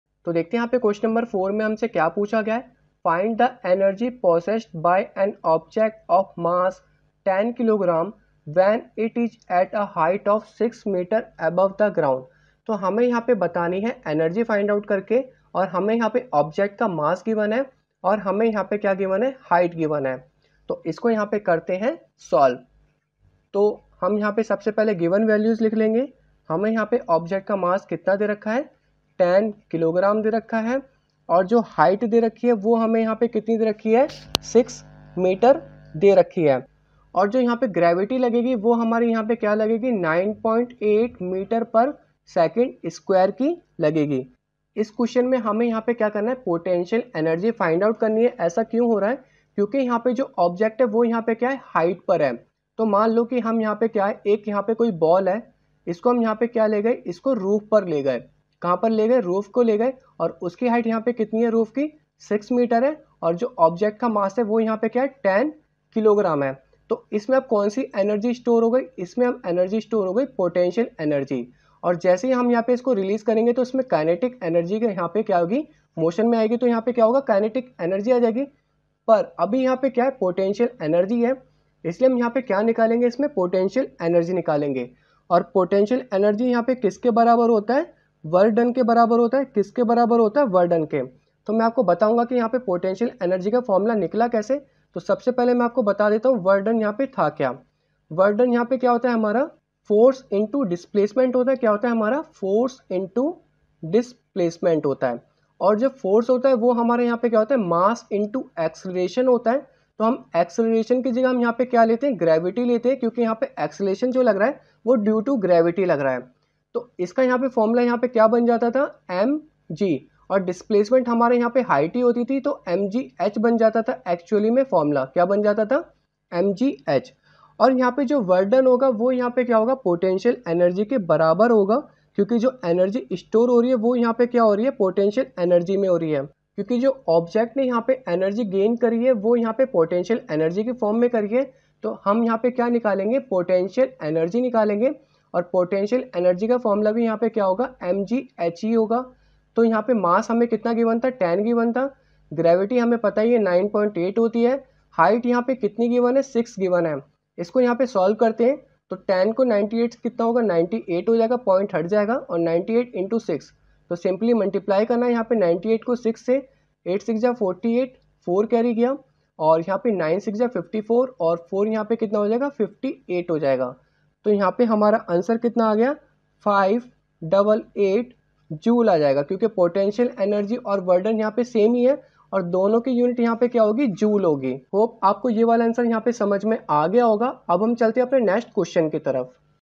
Find the energy possessed by an object of mass 10 kg when it is at a height of 6 m above the ground.
Q.4 Find the energy possessed by an object of mass 10 kg when it is at a height of 6 m above the ground.